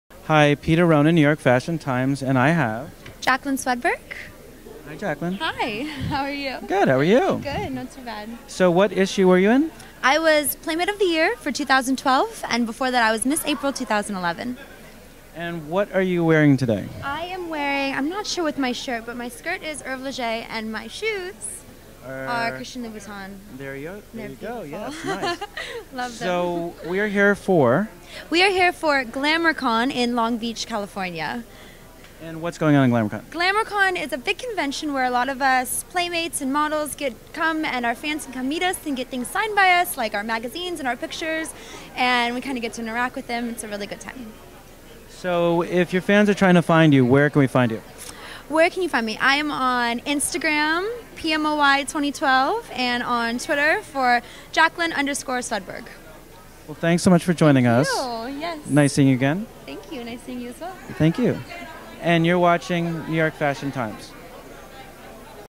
Playboy Playmate of the Year 2012 Jaclyn Swedberg video at Glamourcon 2013, Long Beach, CA
Playboy Playmate of the Year 2012 Jaclyn Swedberg is interviewed by NewYorkFashionTimes.com at Glamourcon 2013, Long Beach, CA on  August 24, 2013.
@Jaclyn_Swedberg Miss April 2011

Jose R. Sousateles Jr. -- Camera/Post